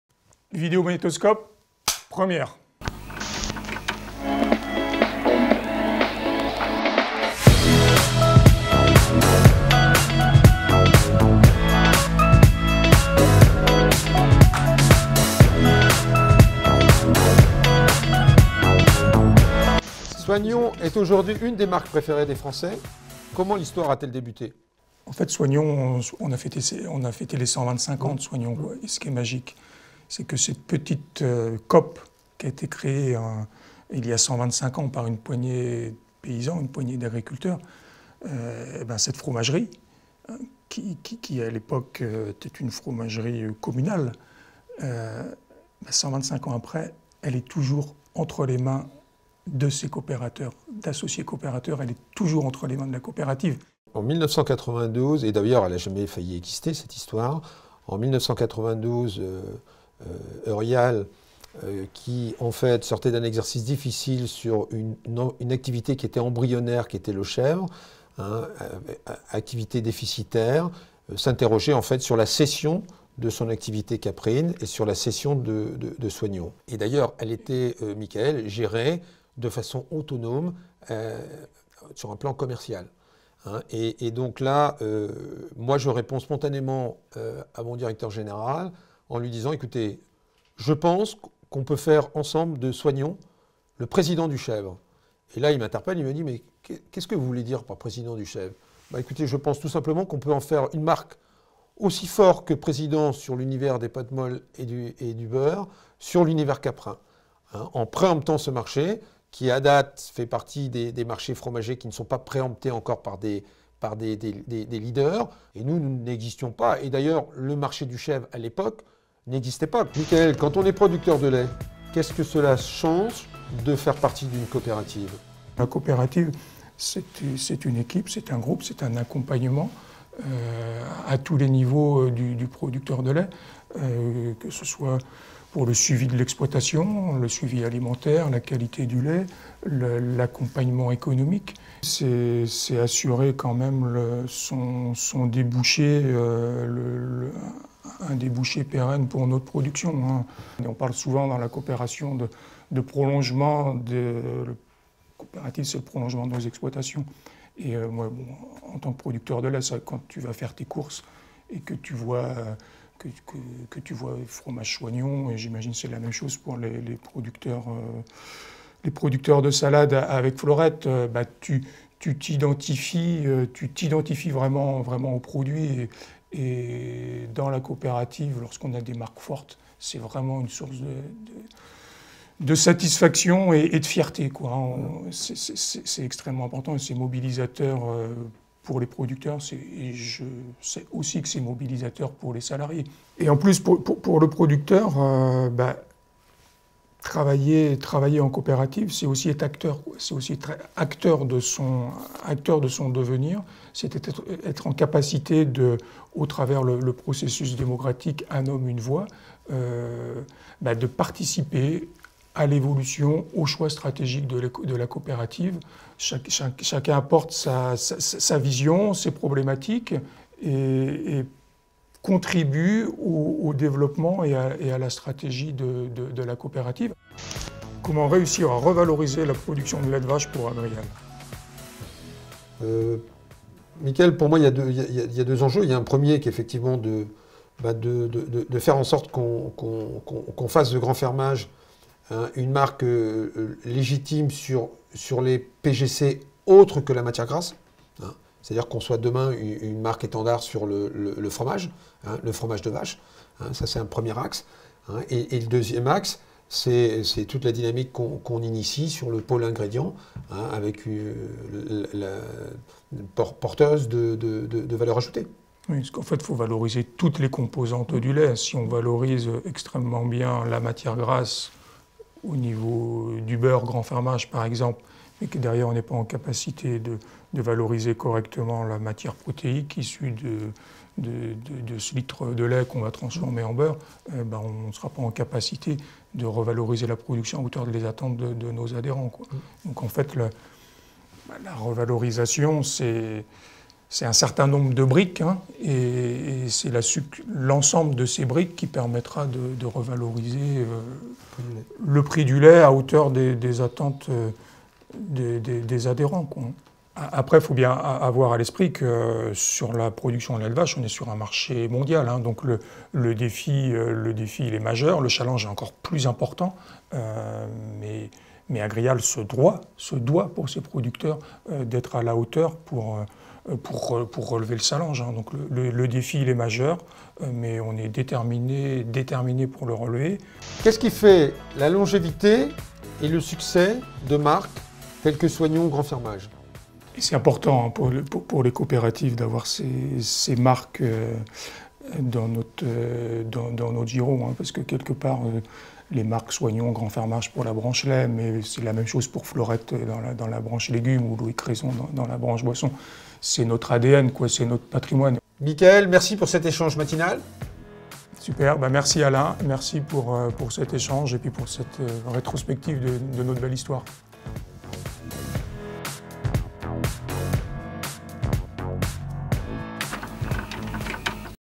[Co-operative Rewind] Episode 5: Some anecdotes about the history of milk
In 2020, Agrial turns 20! Every month, you will find a video retracing a significant event in the history of our Co-operative.

In this 4th episode, Mickaël Lamy Agrial administrator and farmer member in Chênehutte-Trèves-Cunault (49) et Alain Mevel, Retail Sales Manager France, take a look at some facts and anecdotes about the history of milk in the Co-operative.